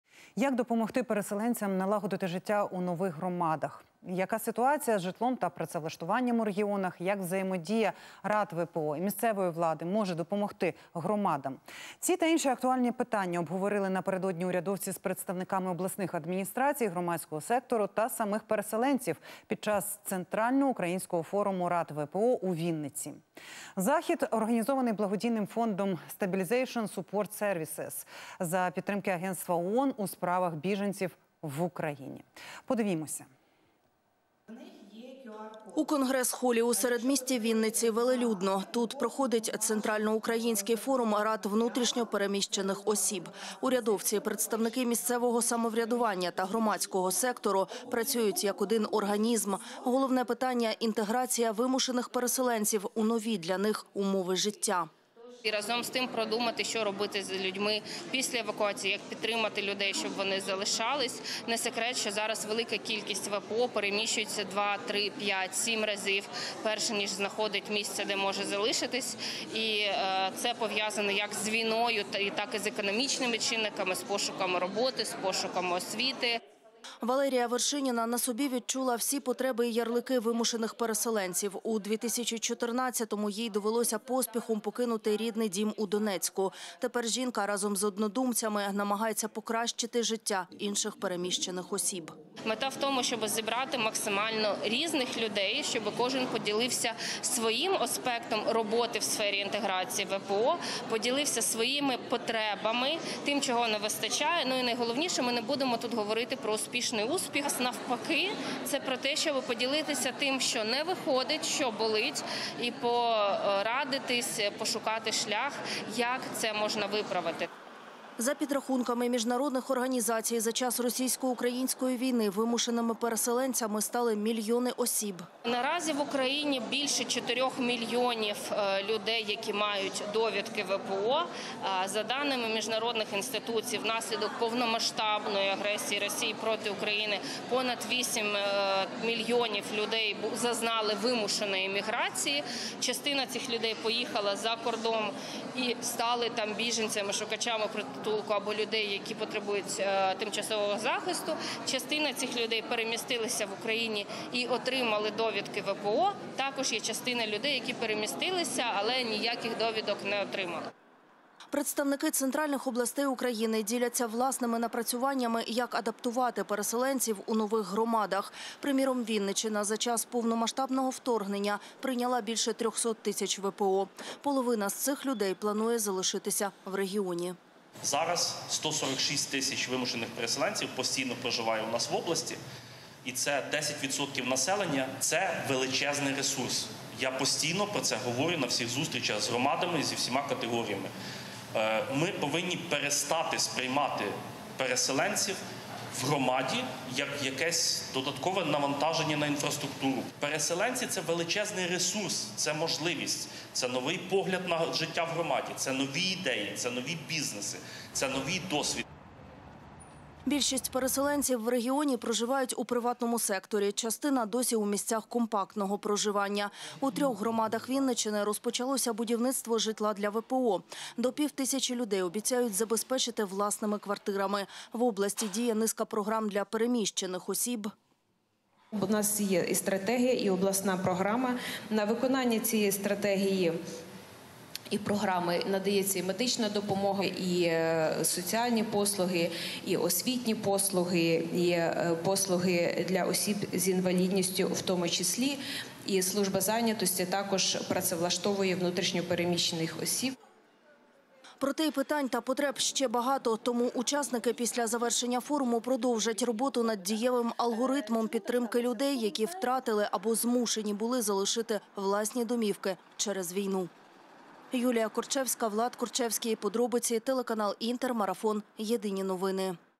У Вінниці відбувся Центральноукраїнський ФОРУМ Рад ВПО
Як допомогти переселенцям налагодити життя у нових громадах? Яка ситуація з житлом та працевлаштуванням у регіонах? Як взаємодія Рад ВПО і місцевої влади може допомогти громадам? Ці та інші актуальні питання обговорили напередодні урядовці з представниками обласних адміністрацій, громадського сектору та самих переселенців під час Центральноукраїнського форуму Рад ВПО у Вінниці. Захід організований благодійним фондом "Стабілізейшен Суппорт Сервісез" за підтримки агентства ООН у справах біженців в Україні. Дивіться.